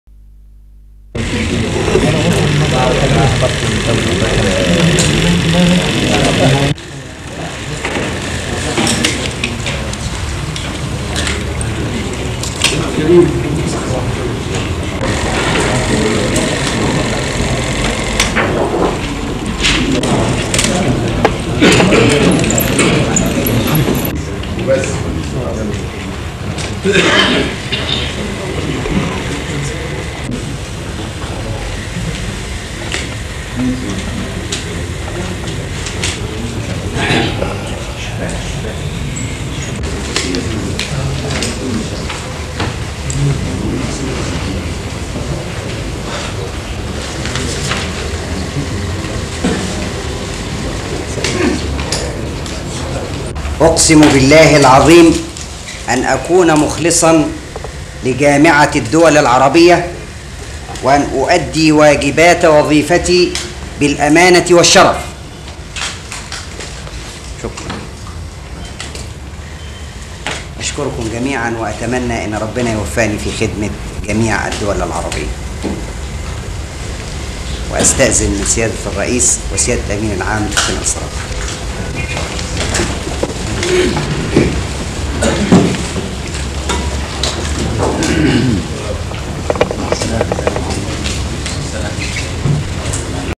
SYND 1-7-71 the arab league meet in extraordinary session in cairo
(30 Jun 1971) The Arab league meet in extraordinary session in Cairo for the swearing-in of the new assistant secretary for military affairs, General Saad El Shazly.